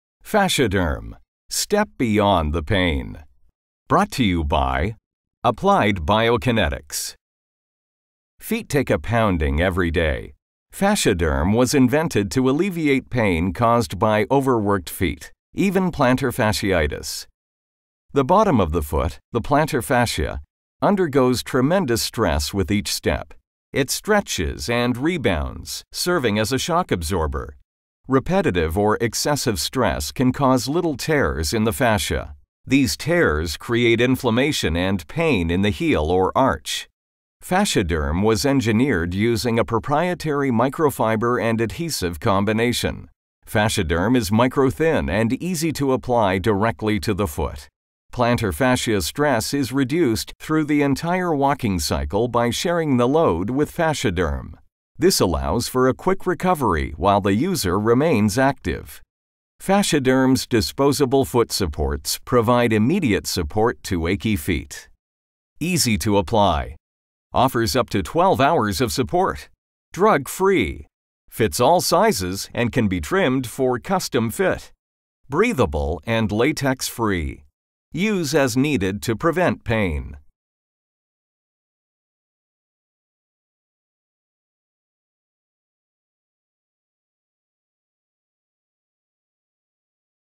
FasciaDerm® - 3D Medical Animation
This product animation was developed for FasciaDerm® Brand Foot Supports which provides pain relief to those with over-used and achey feet. FasciaDerm foot supports are disposable, micro-thin, and bandage-like supports which have been engineered to manage foot strain.